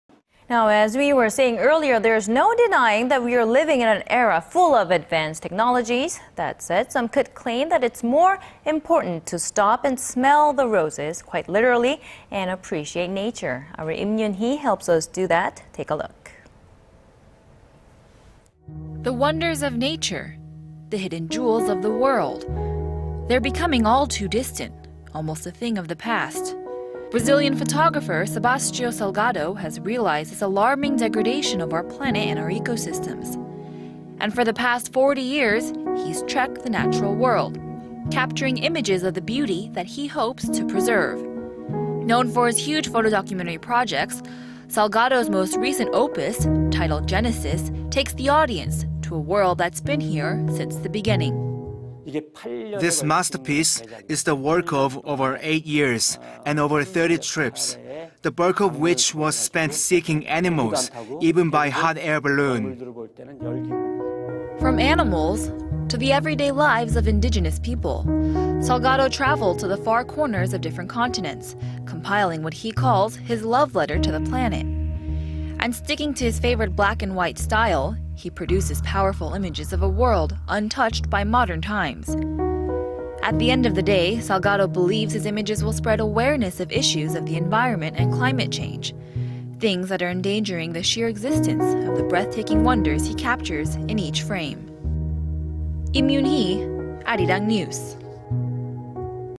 Photography of conservation 환경보전 사진전시
There′s no denying that we′re living in an era full of advanced technologies.
That said... some could claim that it′s more important to stop and smell the roses quite literally, and appreciate nature.
Our Yim Yoonhee helps us do that.
Take a look. The wonders of nature,... the hidden jewels of the world,... they′re becoming all too distant, almost a thing of the past.
Brazilian photographer Sebastiao Salgado has realized this alarming degradation of our planet and our ecosystems,... and for the past 40 years, he′s trekked the natural world, capturing images of the beauty that he hopes to preserve.
Known for his huge photodocumentary projects, Salgado′s most recent opus titled "Genesis" takes the audience to a world that′s been here since the beginning.
[KOREAN]
"This masterpiece is the work of over 8 years, and over 30 trips,... the bulk of which was spent seeking animals, even by hot air balloon."
From animals,... to the every day lives of indigenous people, Salgado traveled to the far corners of different continents, compiling what he calls his "love letter to the planet."
And sticking to his favored black and white style, he produces powerful images of a world untouched by modern times.
At the end of the day, Salgado believes his images will spread awareness of issues of the environment and climate change,... things that are endangering the sheer existence of the breathtaking wonders he captures in each frame.
Yim Yoon-hee, Arirang News.